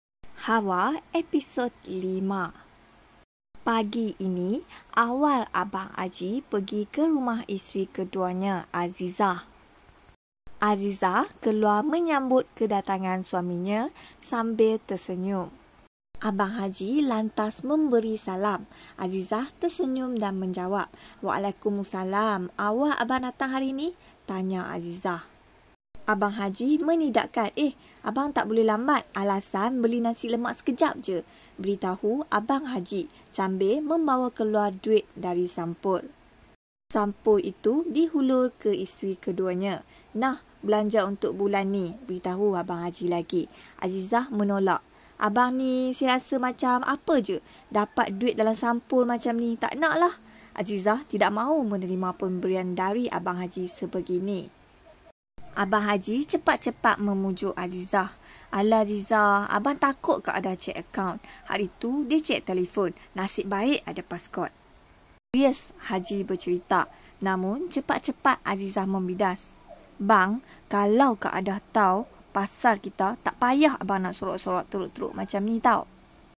HAWA EPS 5 - ABG AJI CURI2 PERGI RUMAH ISTERI KEDUA..TIPU KATA KELUAR BELI NASI LEMAK 😂
Haiihh betul kan orang kata zaman sekarang ni... Macam2 betul...
Cerita ni, dalam real life pun ada jadi...
So kita layan kan je...